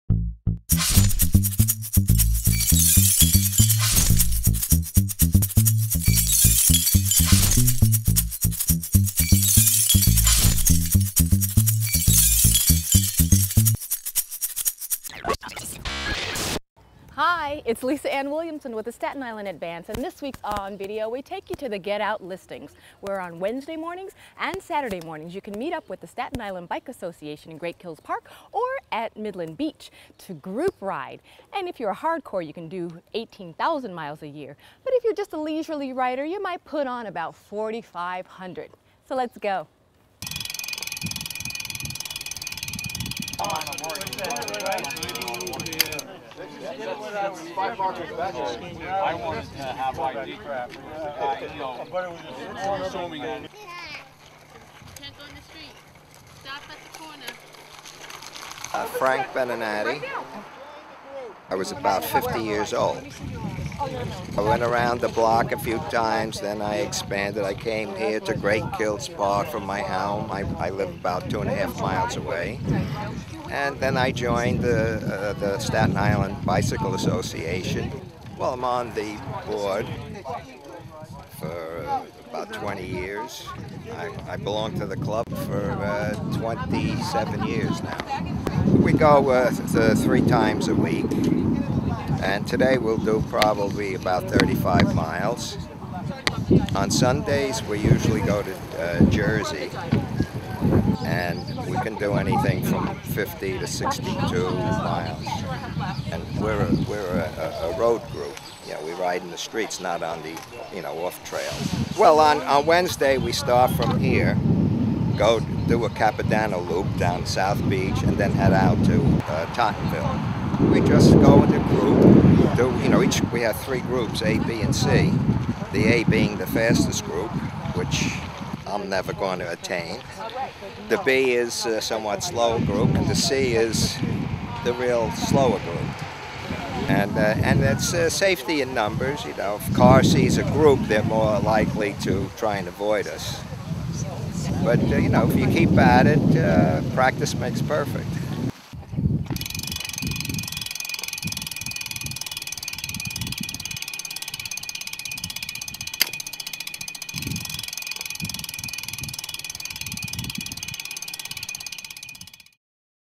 Miles to pedal
Staten Island Bicycling Association members put thousands of miles on bikes riding through town.